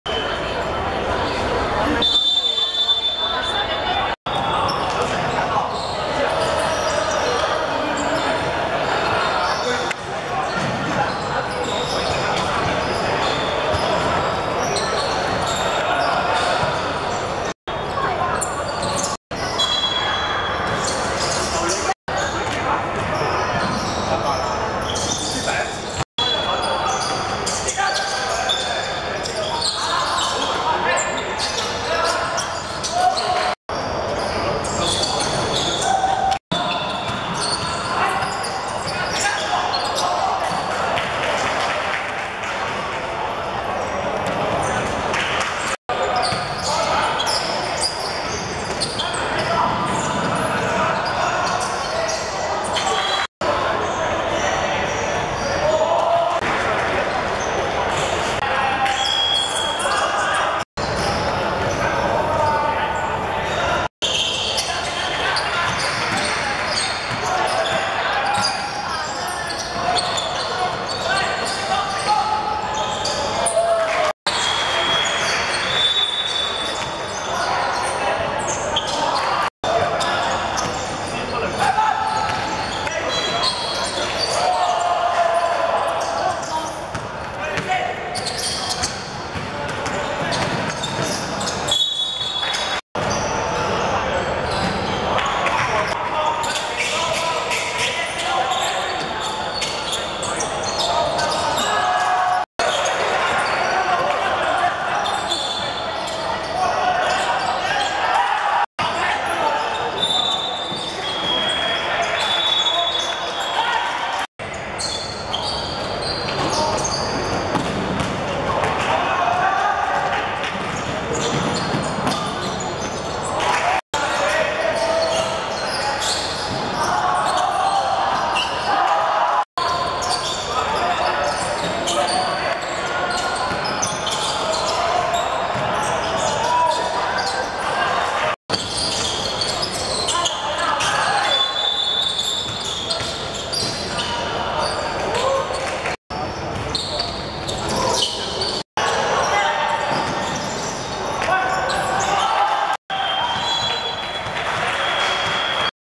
保良局何玉清vs滬江維多利亞(2015.6.28.學界籃球馬拉松男子分組賽第二圈)精華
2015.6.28.港灣道體育館 10:45 保良局何玉清(紫) 10:26 滬江維多利亞(白)